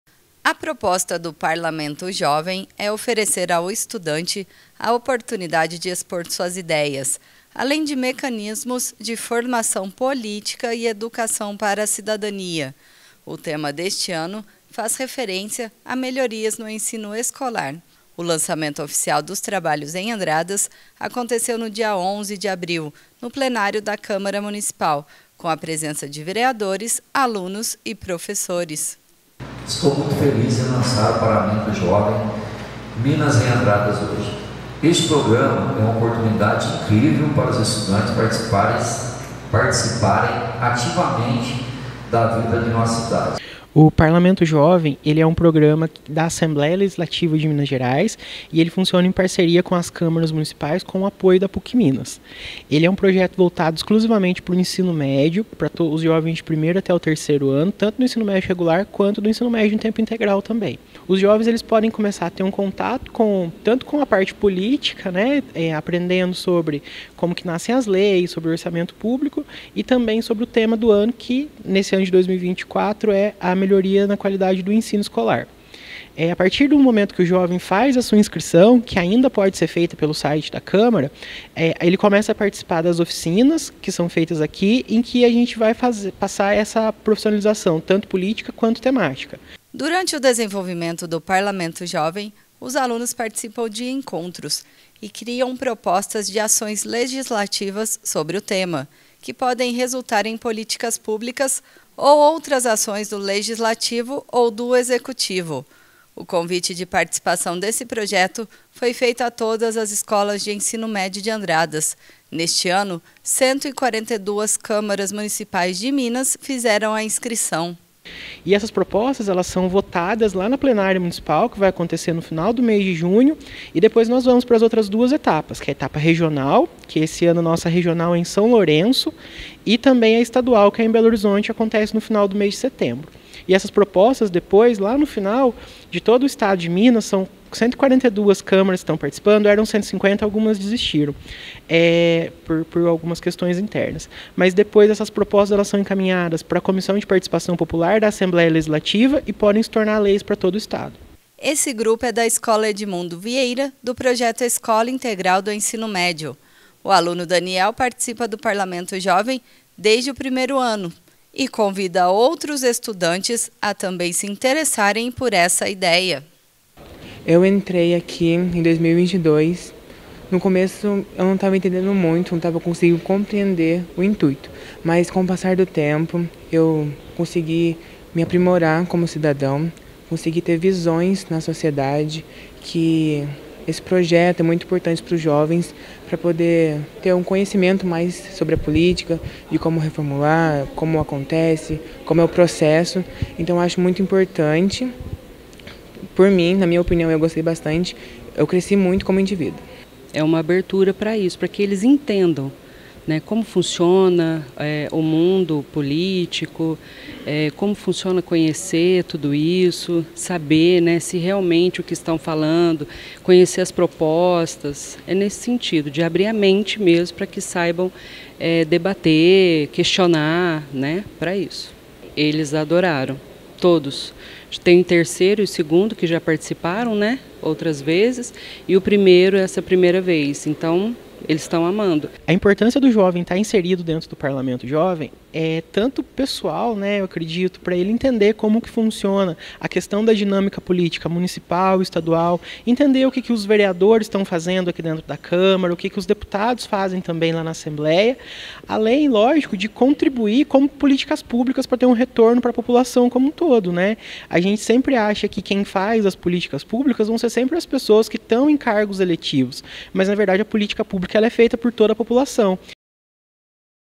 Estudantes do Ensino Médio participam do lançamento do Parlamento Jovem
Na quinta-feira, dia 11 de abril, aconteceu o lançamento do Parlamento Jovem na Câmara Municipal de Andradas. Essa é uma iniciativa de formação política para estudantes do ensino médio.